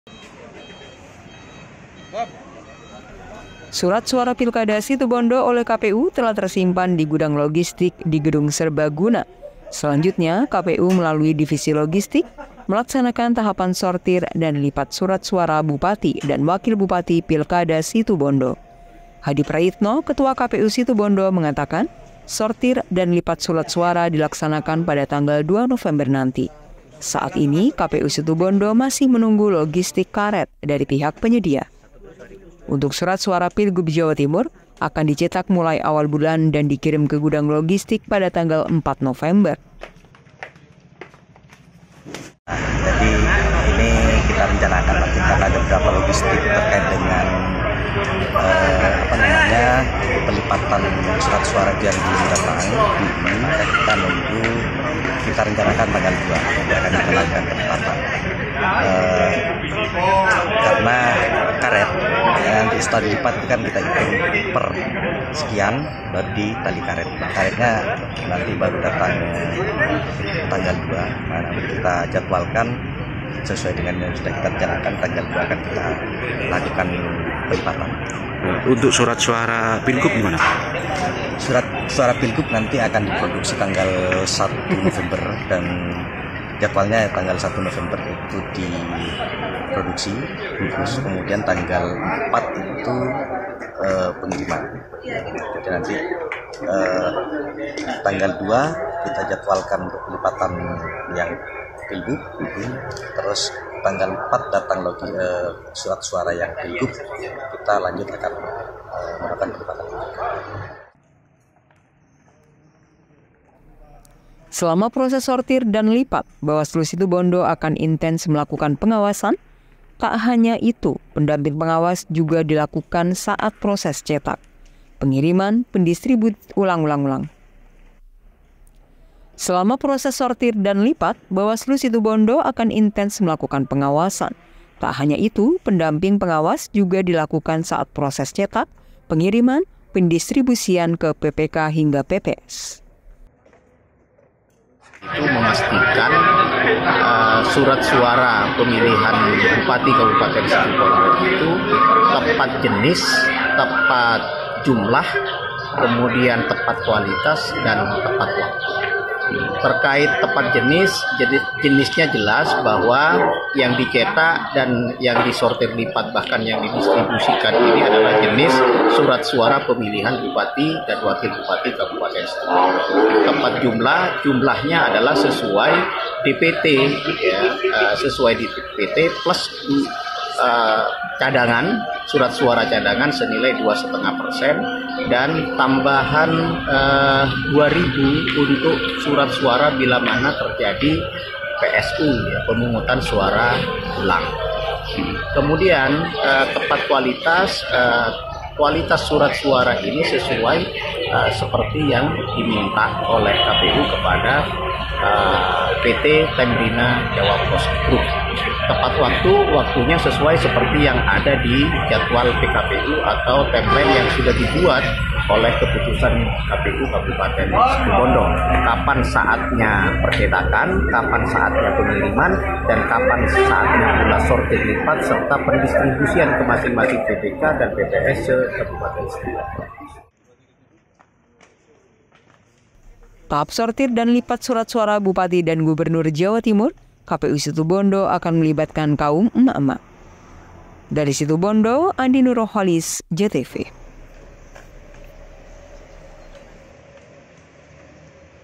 2 NOVEMBER, KPU SITUBONDO LAKSANAKAN SORTIR DAN LIPAT SURAT SUARA
SURAT SUARA PILKADA SITUBONDO OLEH KPU TELAH TERSIMPAN DI GUDANG LOGISTIK DI GEDUNG SERBA GUNA . SELANJUTNYA KPU MELALUI DIVISI LOGISTIK MELAKSANAKAN TAHAPAN SORTIR DAN LIPAT SURAT SUARA BUPATI DAN WAKIL BUPATI PILKADA SITUBONDO.